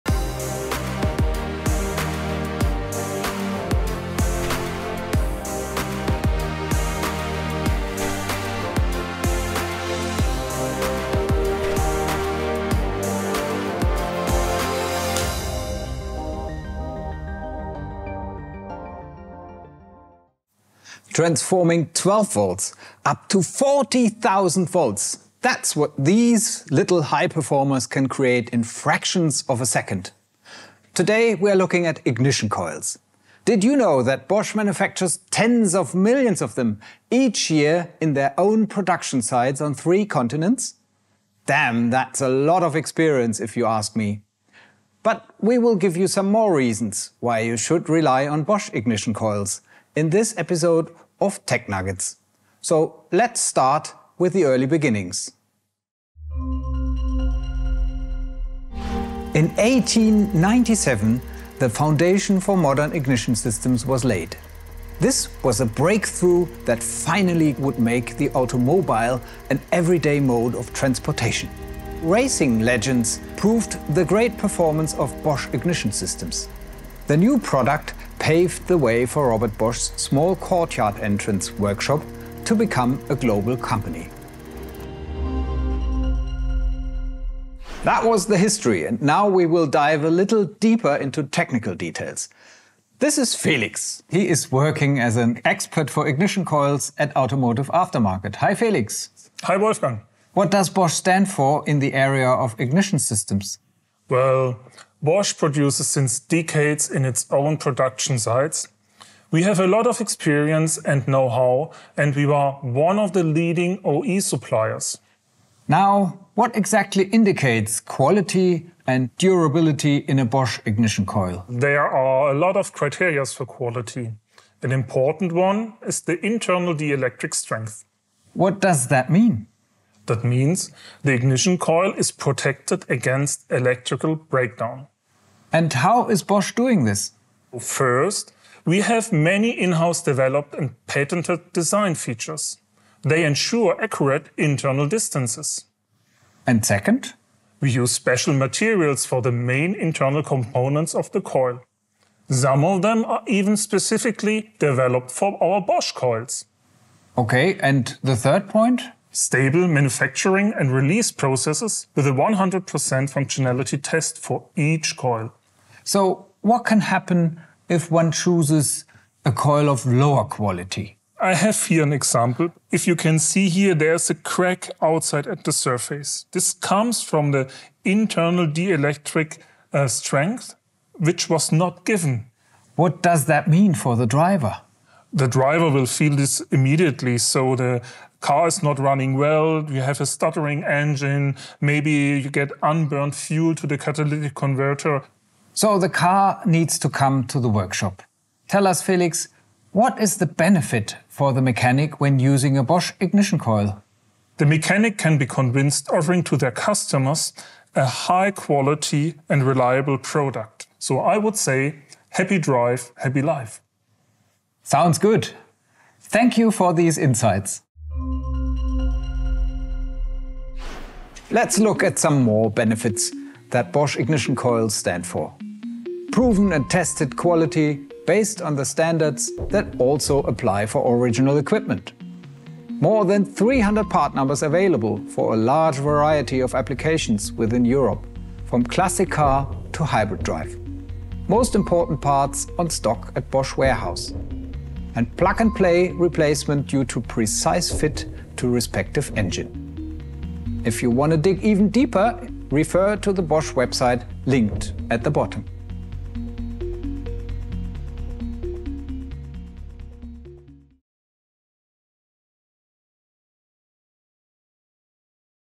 Bosch TechNuggets: Ignition coils - Automotive. Knowledge. Explained
This episode of Bosch TechNuggets is all about the Bosch ignition coil and where its high quality and durability derives from. It will provide you interesting technical details about what Bosch stands for in the area of ignition systems. You will find out more about the internal dielectric strength and what makes Bosch coils high performing. A look back at Bosch history, an interesting expert interview and all advantages of Bosch coils at a glance.
Watch and find out why workshops can rely on Bosch ignition coils!